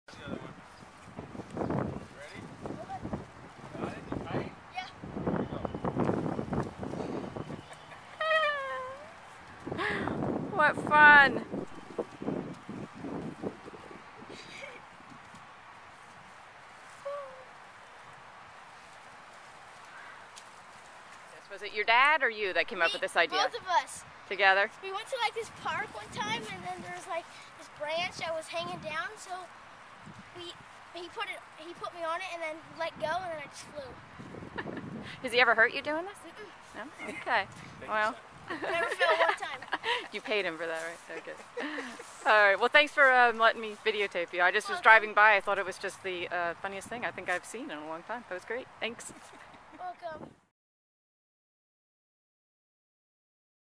Swinging_in_the_Trees_in_Los_Altos
Keeping children occupied can be viewed as a mundane job, or you can be like this creative Los Altos dad Stephen Nations, and find  entertainment with the simplest things right in your own backyard. I was driving by the south side of Whaley park on my way to preview a home when I spied this child flying out of a tree. I had to stop and get this on tape for the blog. Thanks to the Nations family for allowing me to join in on the fun.

I Know Long Beach and I Love Los Altos!